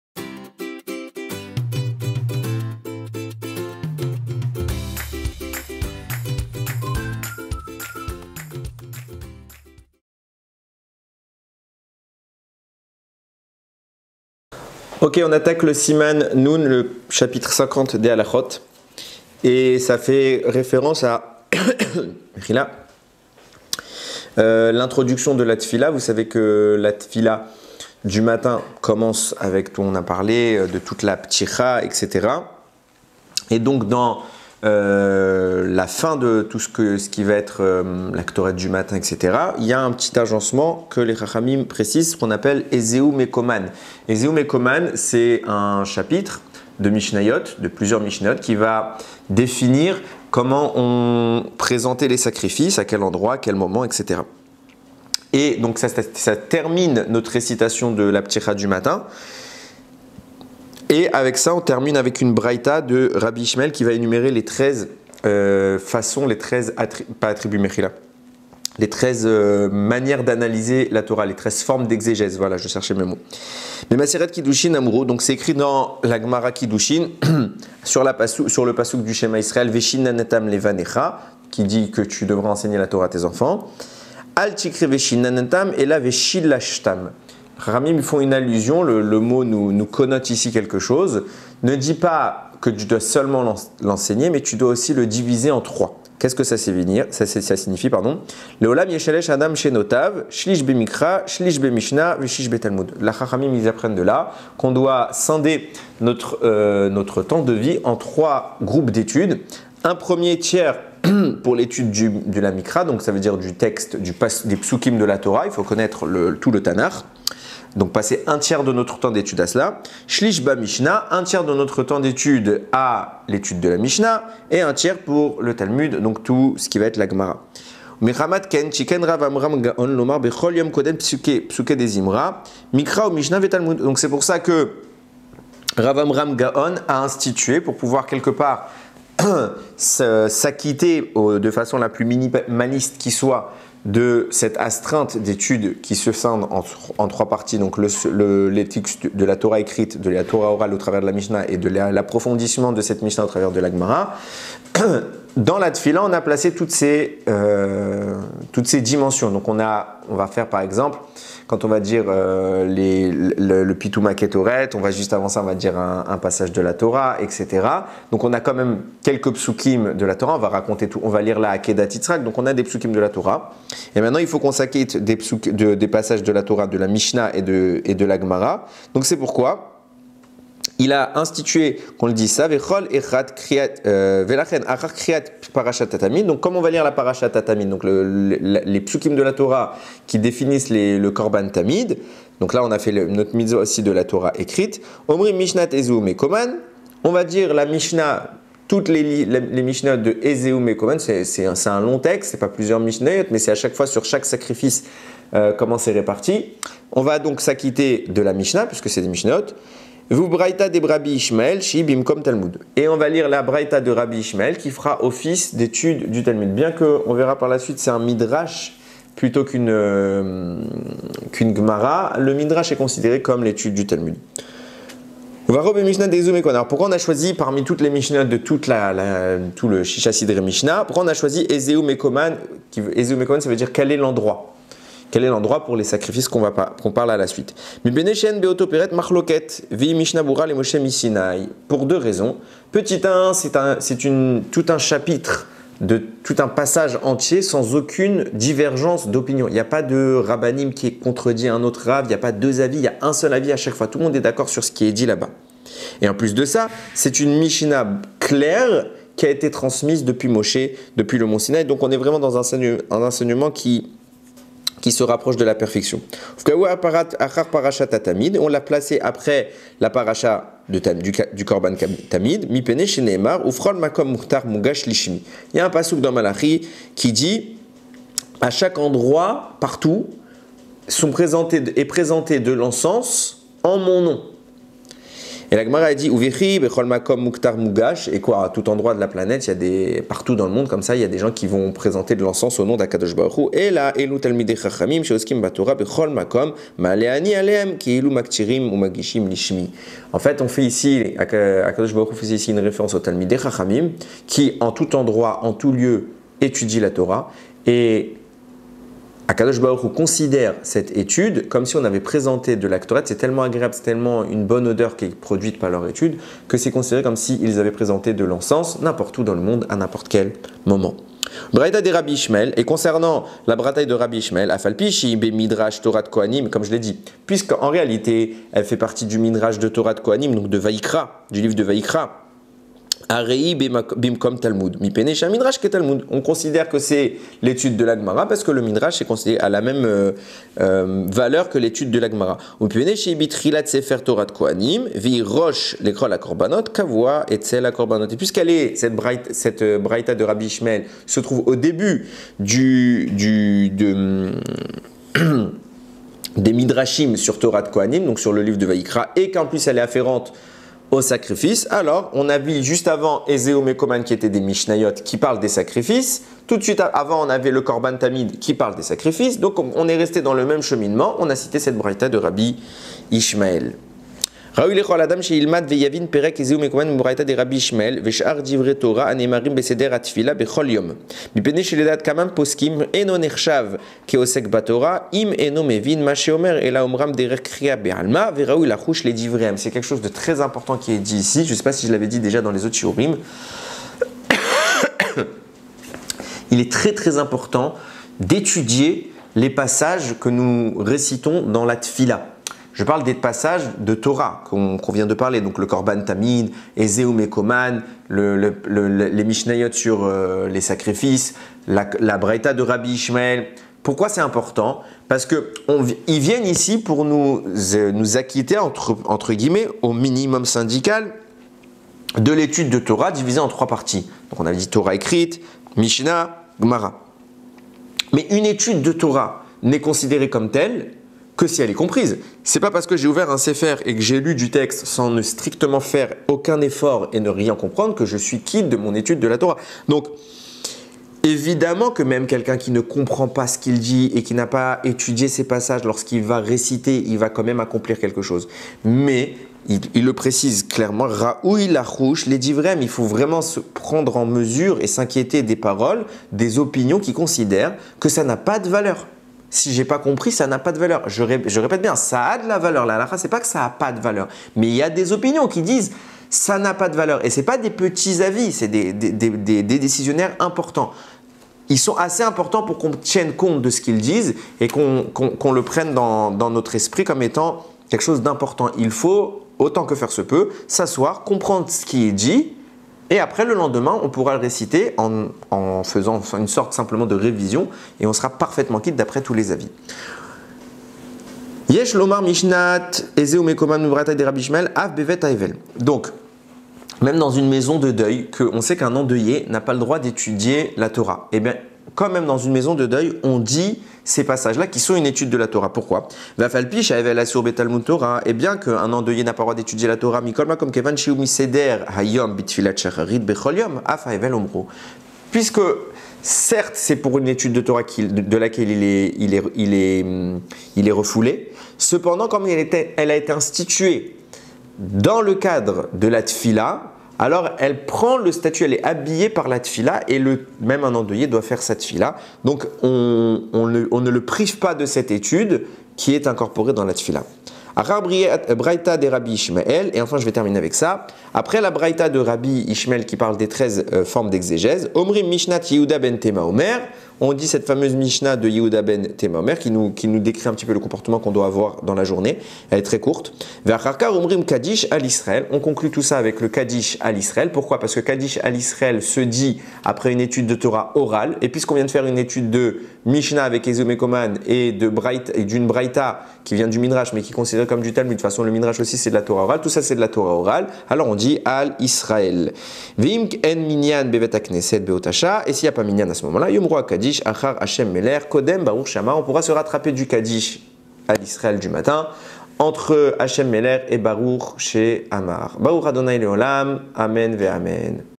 Quelques minutes d'Halakha: Les premiers passages de la prière
Pourquoi récite-t-on Ézéhou Mékomane ainsi que le braïta de Rabbi Yichmaël.
Le besoin d'étudier chaque sujet de la Torah.
Comprendre nos études durant la prière.